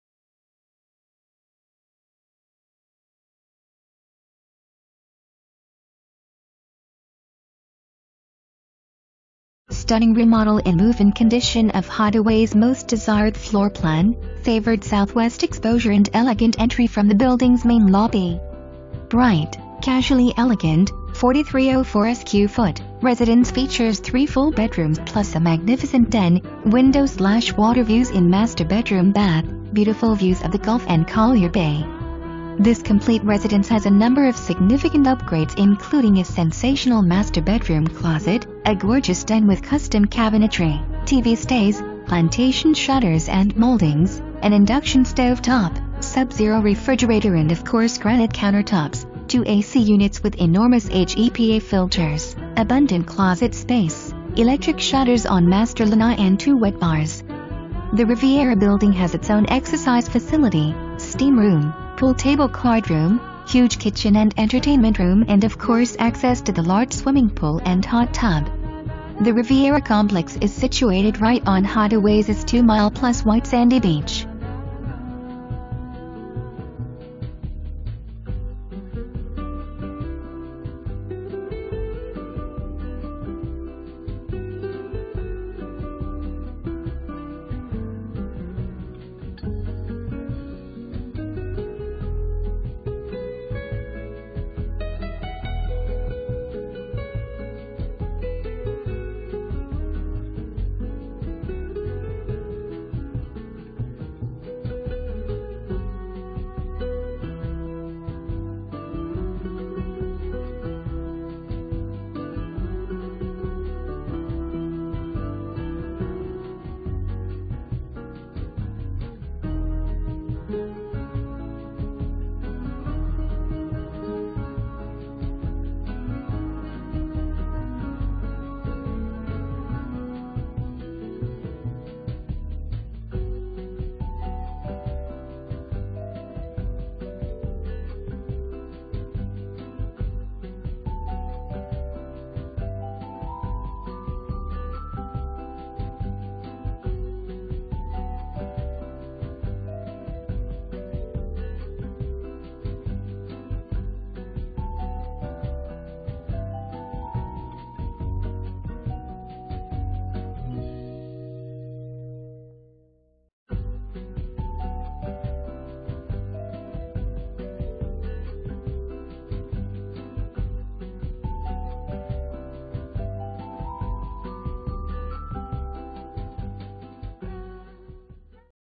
Welcome to 4000 Royal Marco Way #424!
4000 Royal Marco Way #424

STUNNING REMODEL IN MOVE-IN CONDITION OF HIDEAWAY'S MOST DESIRED FLOOR PLAN, FAVORED SOUTHWEST EXPOSURE AND ELEGANT ENTRY FROM THE BUILDING'S MAIN LOBBY! BRIGHT,CASUALLY ELEGANT, 4304 SQ/FT, RESIDENCE FEATURES 3 FULL BEDROOMS PLUS A MAGNIFICENT DEN, WINDOWS /WATER VIEWS IN MASTER BEDROOM/BATH, BEAUTIFUL VIEWS OF THE GULF AND COLLIER BAY. THIS COMPLETE RESIDENCE HAS A NUMBER OF SIGNIFICANT UPGRADES INCLUDING A SENSATIONAL MASTER BEDROOM CLOSET, A GORGEOUS DEN WITH CUSTOM CABINETRY (TV STAYS), PLANTATION SHUTTERS & MOULDINGS, AN INDUCTION STOVE TOP, SUB-ZERO REFRIGERATOR AND OF COURSE GRANITE COUNTER TOPS, TWO A/C UNITS WITH ENORMOUS HEPA FILTERS, ABUNDANT CLOSET SPACE, ELECTRIC SHUTTERS ON MASTER LANAI AND TWO WET BARS. THE RIVIERA BUILDING HAS ITS OWN EXERCISE FACILITY, STEAM ROOM, POOL TABLE/CARD ROOM, HUGE KITCHEN AND ENTERTAINMENT ROOM AND OF COURSE ACCESS TO THE LARGE SWIMMING POOL AND HOT TUB. THE RIVIERA COMPLEX IS SITUATED RIGHT ON HIDEAWAYS'S 2 MILE+ WHITE SANDY BEACH

Presented By:
The Sandlin Team, RE/MAX Affinity Plus